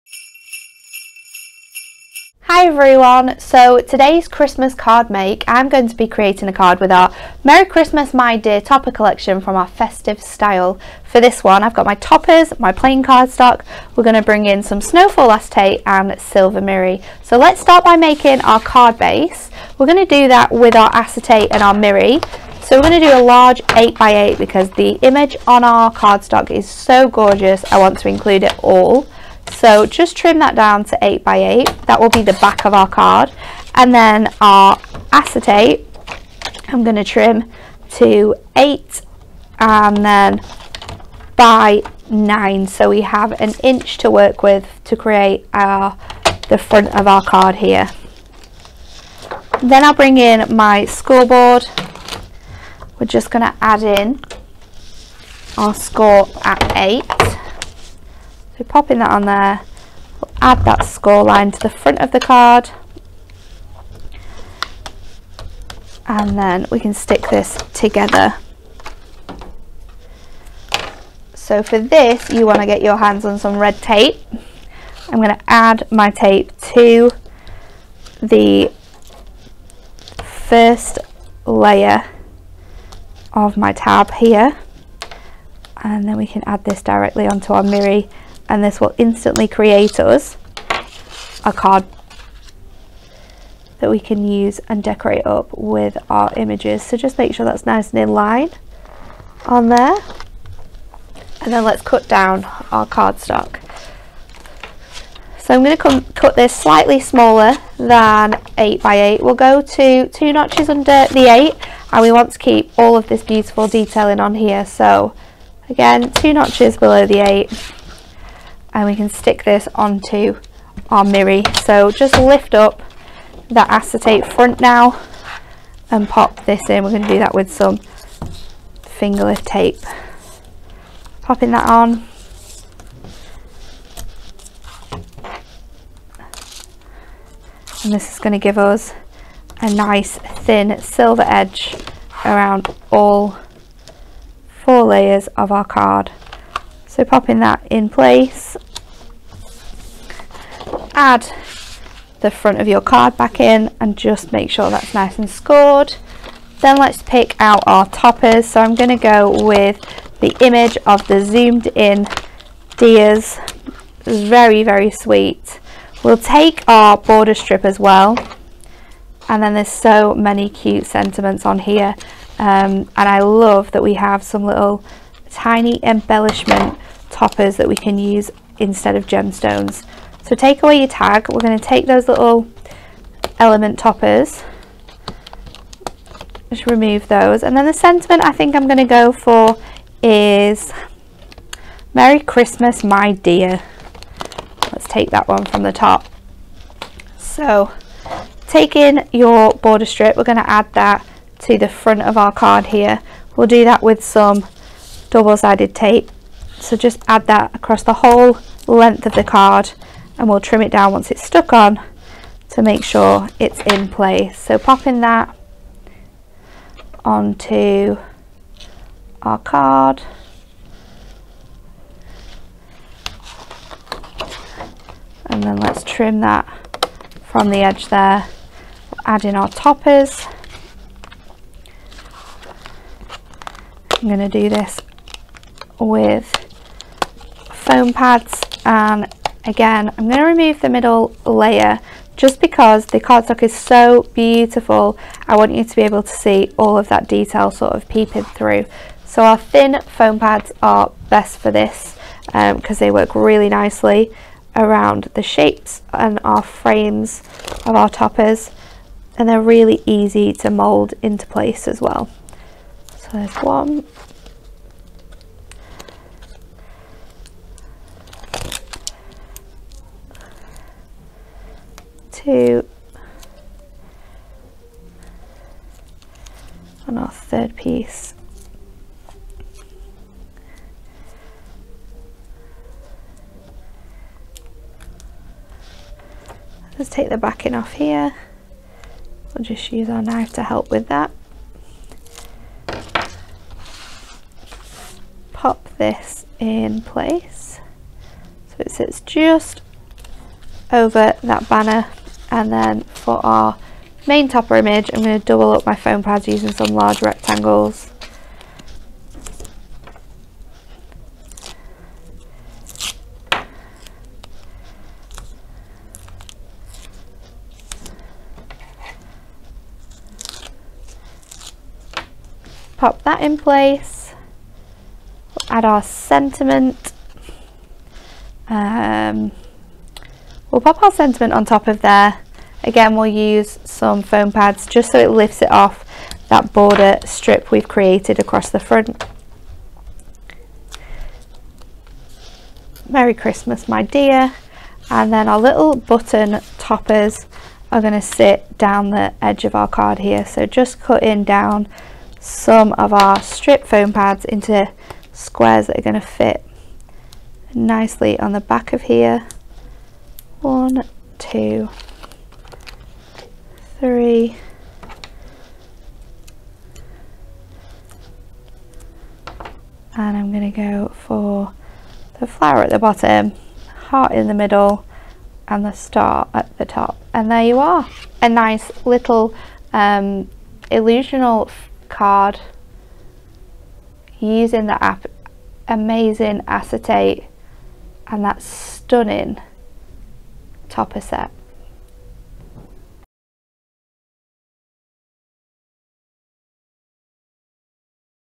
How To: Merry Christmas My Deer
Do you want to make a Christmas card with style? Follow Natalie's step-by-step tutorial, using the Festive Style Collection from our Blockbuster 2022!